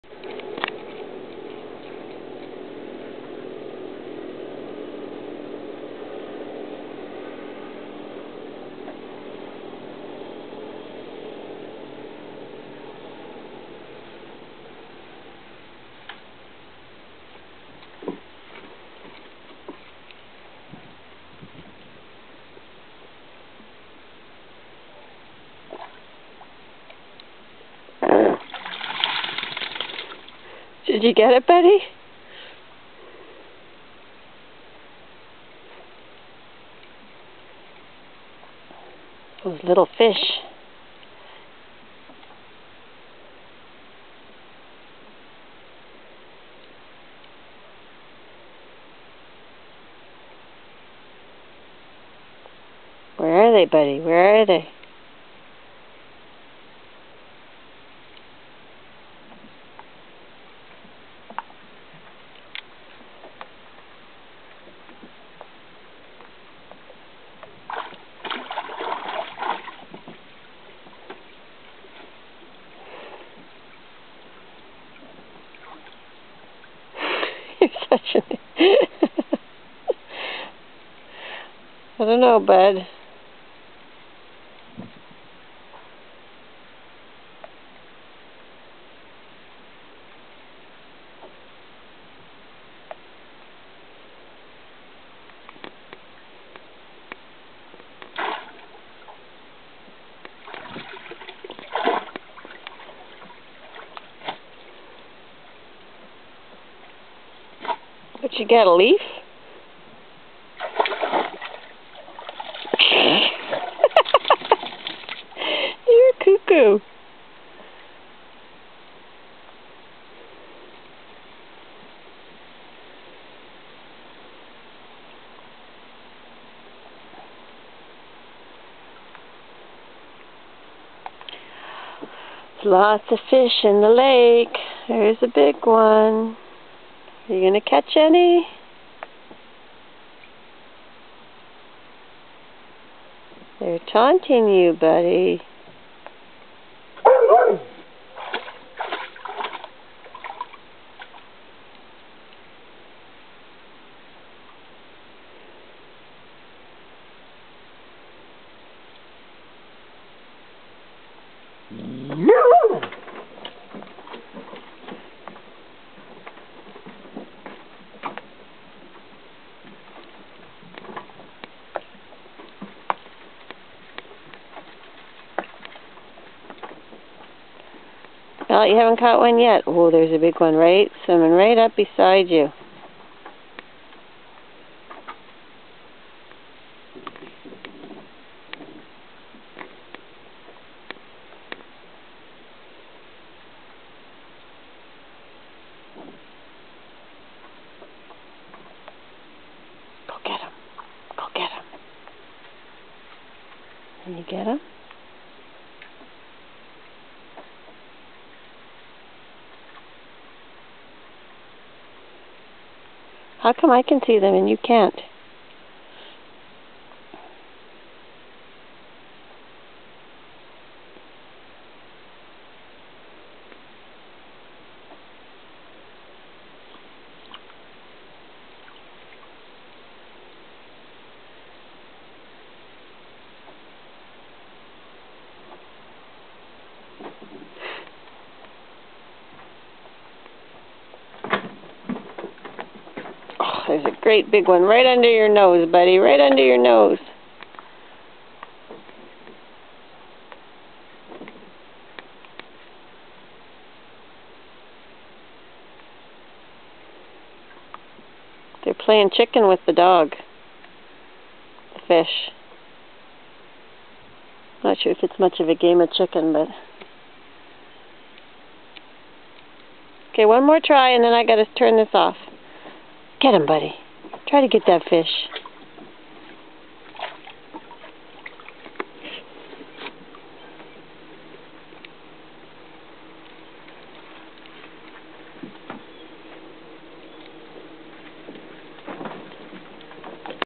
Buddy the Airedale going fishing
Buddy the Airedale and his new favorite sport - fishing